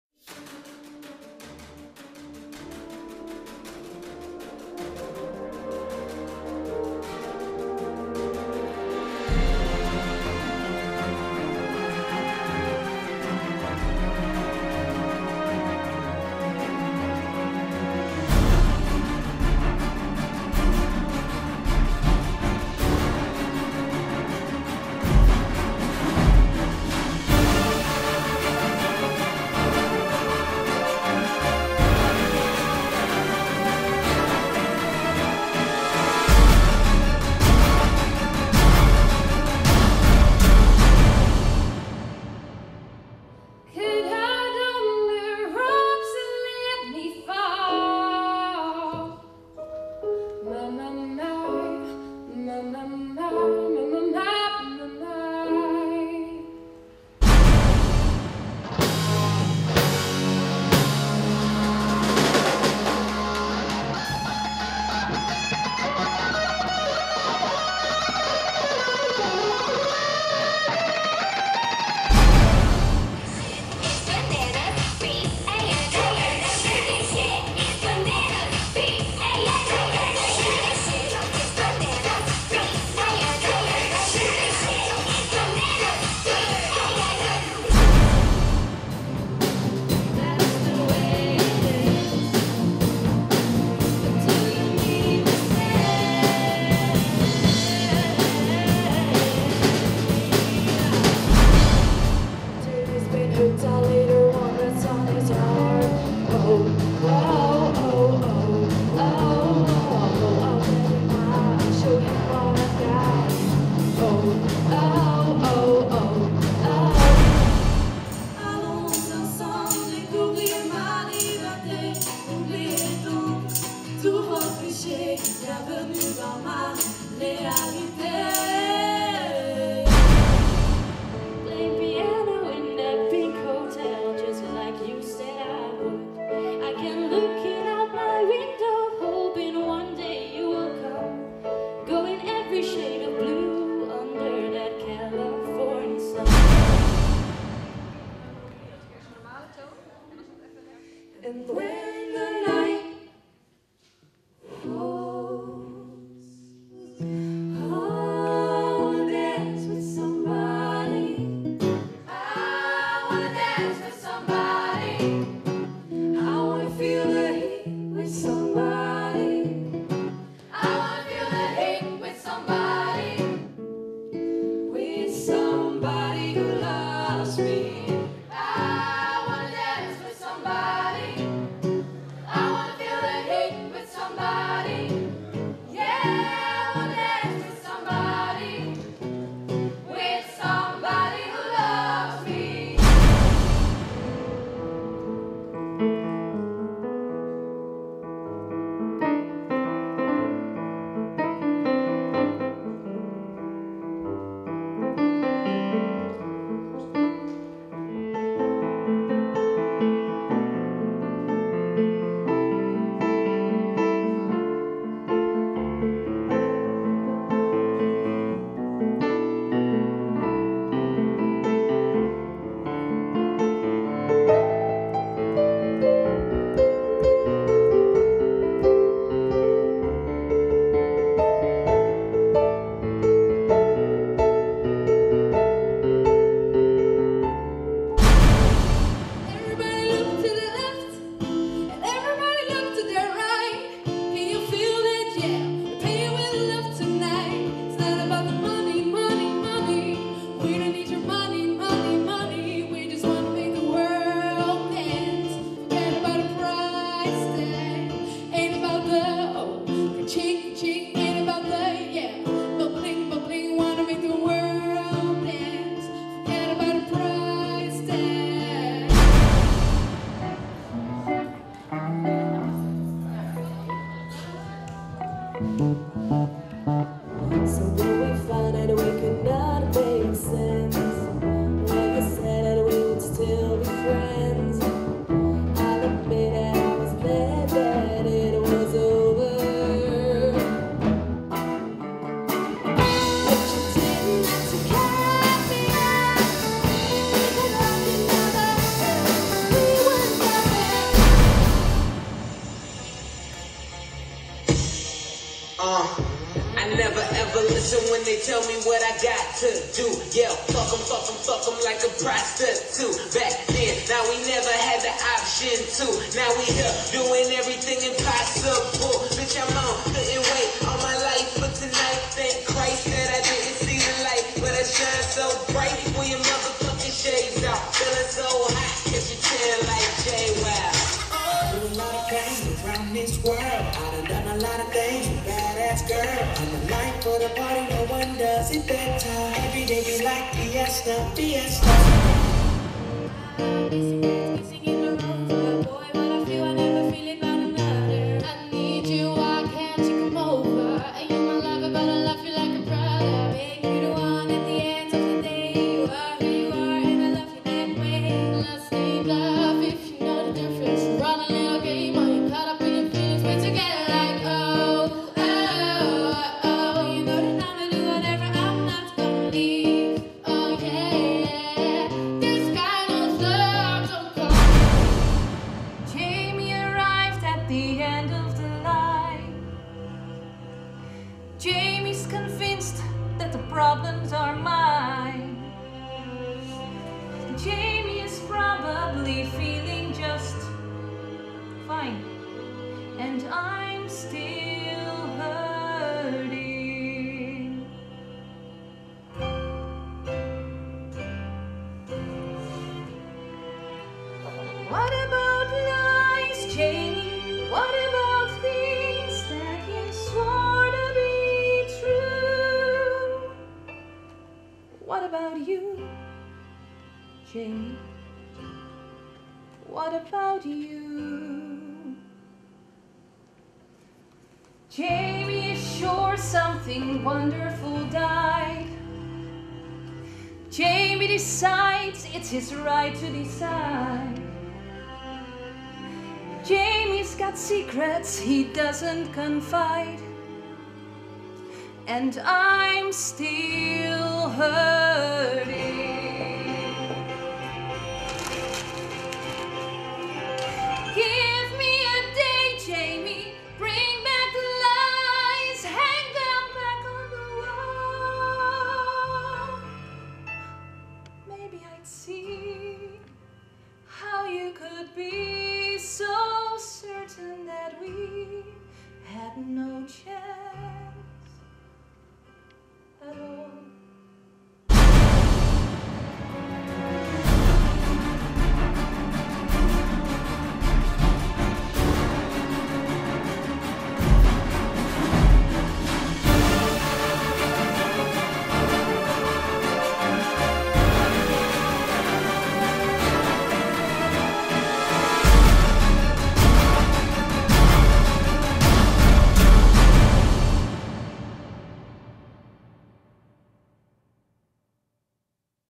Trailer GSG Leo Vroman Night of Dreams 2012
Op donderdag 5 april a.s. vind de jaarlijkse "Night of Dreams" plaats in de Goudse Schouwburg, door leerlingen van DGD Leo Vroman.

Hier alvast een voorproefje van wat de bezoeker te wachten staat.

Trailer gemaakt door:  John van Kleef - Artistic Slolutions